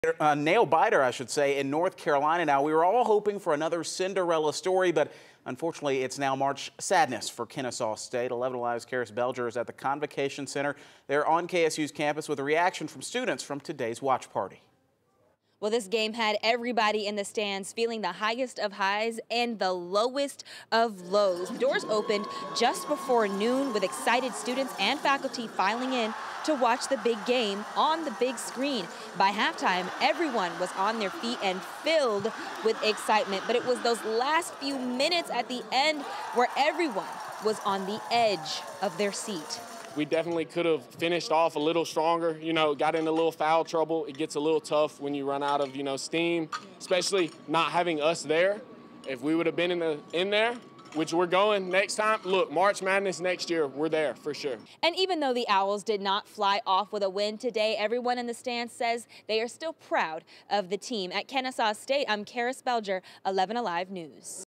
Kennesaw State fans support Owls in first NCAA Tournament game
The game had fans on the edge of their seats.